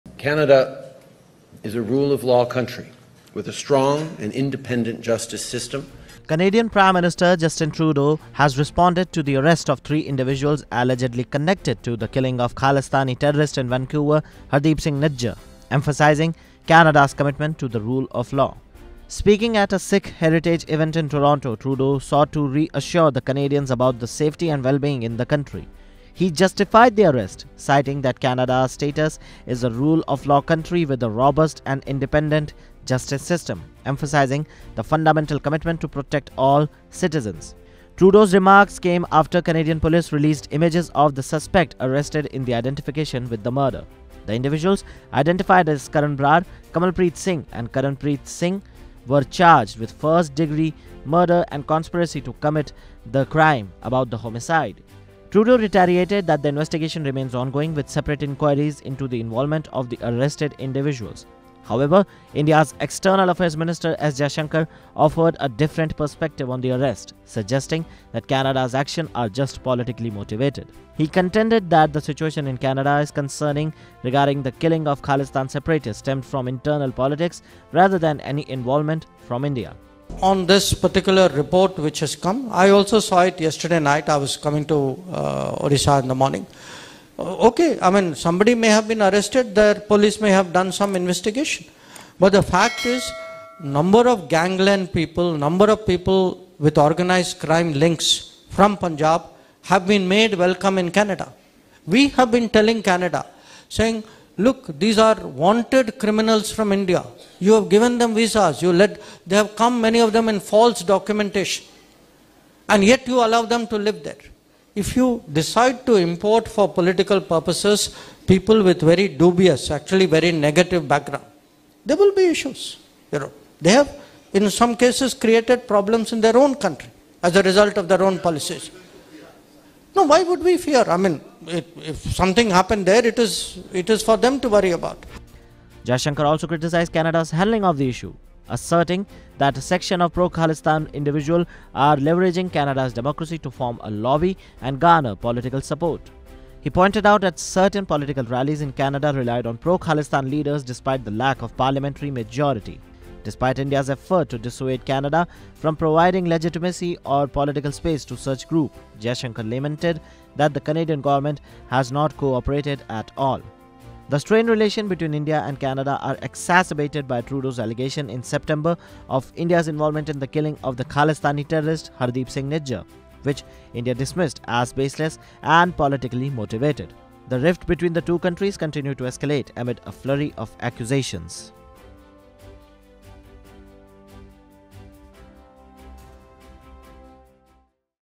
Justin Trudeau Provokes India Again; Flaunts Arrest of 3 Indians In Nijjar Killing Case | Oneindia
Canadian Prime Minister Trudeau assured Sikh Canadians of safety after arrests linked to a Khalistani activist's killing. He emphasized Canada's adherence to the rule of law. However, India's External Affairs Minister Jaishankar criticized Canada's handling, deeming it politically motivated. He highlighted India's strained ties with Canada over allegations of Indian involvement, which India dismissed as baseless.